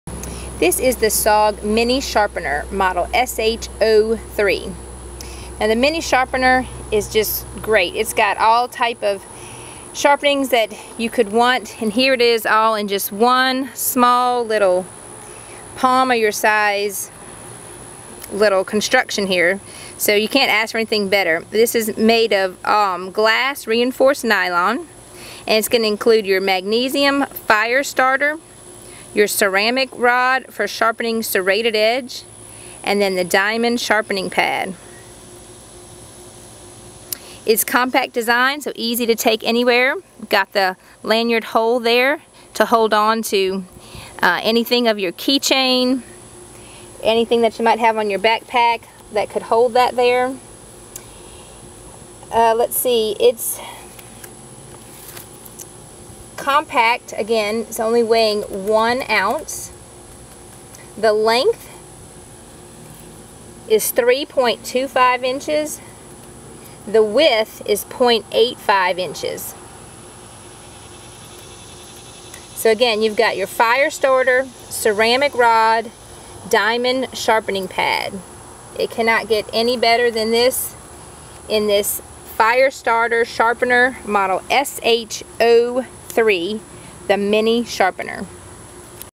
SOG Mini Sharpener wit Fire Starter SH03 - video demo
The SOG Firestarter/Sharpener SH03 is an ingenious design. It has a flat diamond sharpening surface on one side for use with plain edges. One edge of the device has a ceramic sharpener for serrated blades while the other sports a ferrocerium rod for fire starting. All of the features are built into a unit small enough to go on a keychain or to be worn on a lanyard. Glass reinforced nylon.